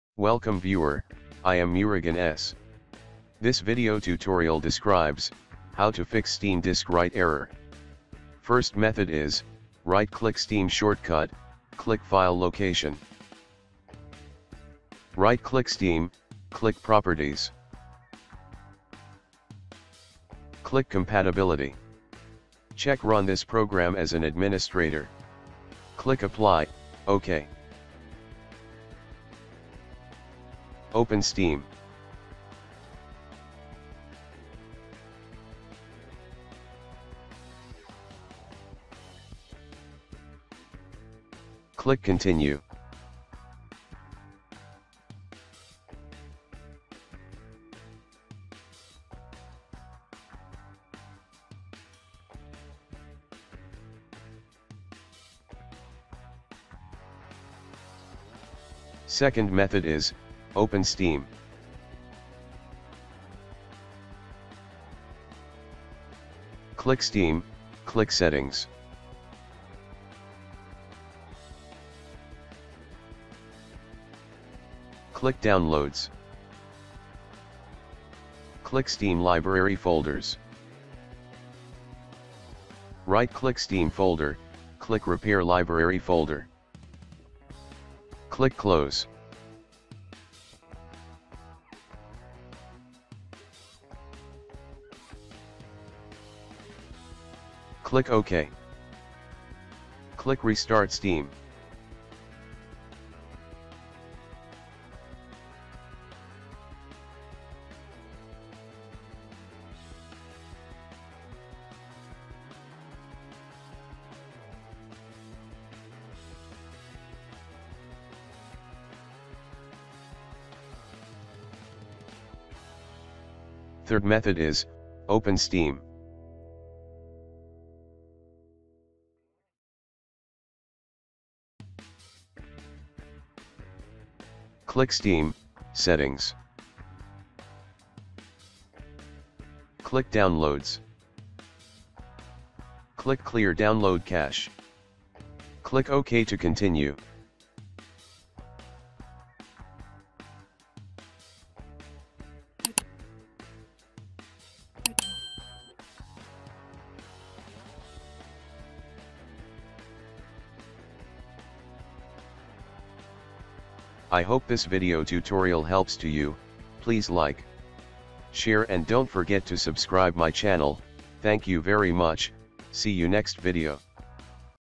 Steam : Fix Steam Disk Write Error [Solved]
This Tutorial Helps to Steam : Fix Steam Disk Write Error [Solved]